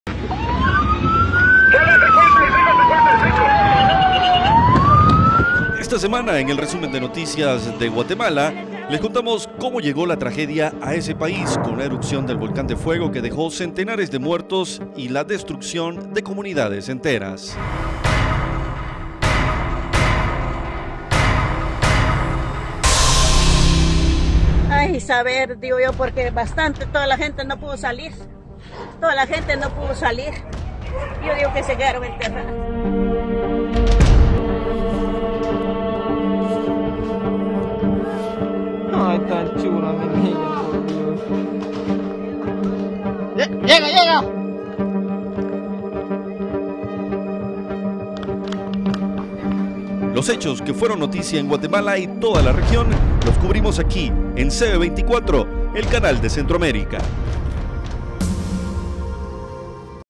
Repasamos lo que fue noticia en el 2018 en Guatemala
En EsNoticiaCentroamérica, repasamos lo que fue noticia en Guatemala durante el 2018.

24 Horas conectados con la información